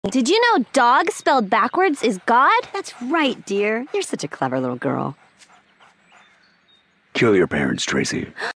Dog Spelled Backwards Is God // New warrior cat ocs // Read description
Characters in order of appearance:
Squirrelkit(tail)(She/Her)
Dovestar (She/They)
Snailsky (She/Her)

Baby gets haunted by a dead cat who has it out for her mom